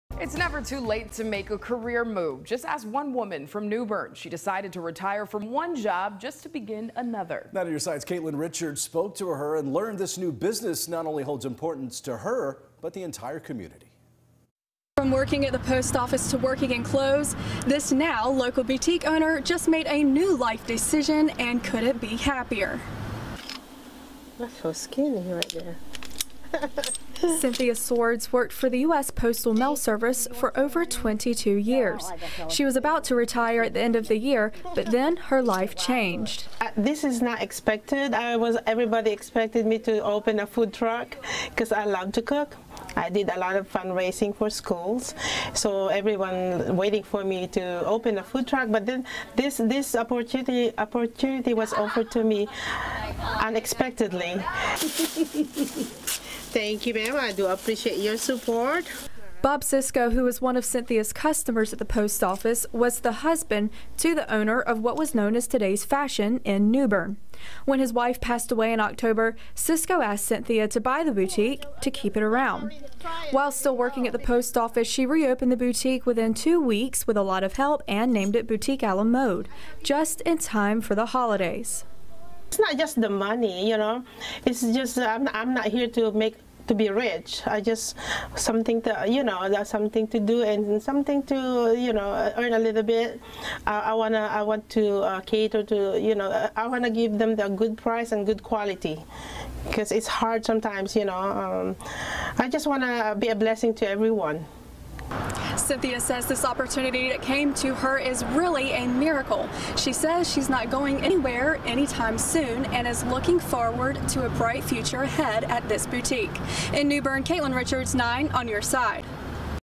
Retired New Bern postal worker gets opportunity to own, operate own shop
Cynthia Swords worked for the US Postal Service for over 22 years. She was about to retire at the end of the year, but then her life changed unexpectedly when she got offered a boutique and the inventory in it.